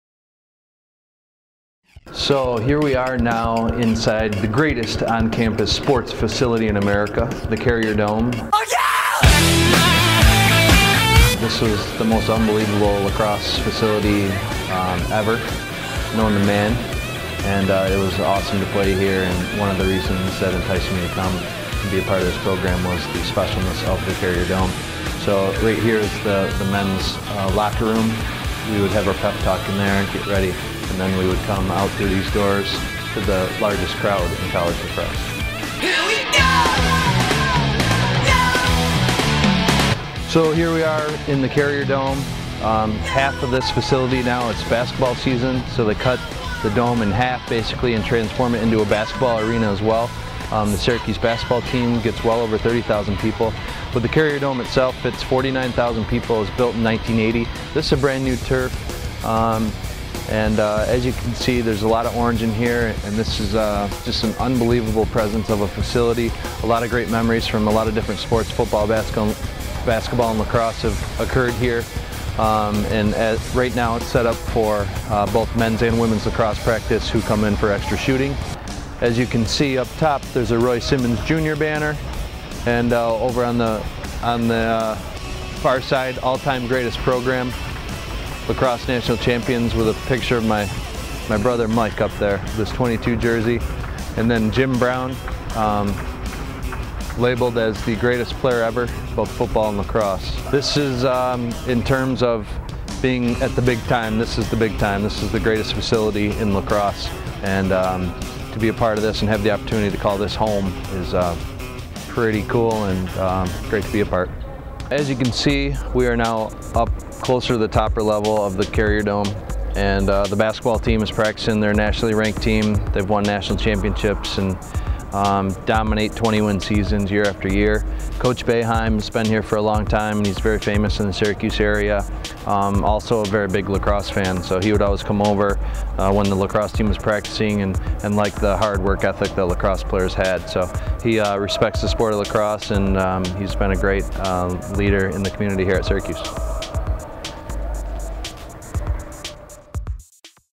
Casey Powell- Syracuse Carrier Dome
Casey Powell gives IL a tour of Syracuse's Carrier Dome.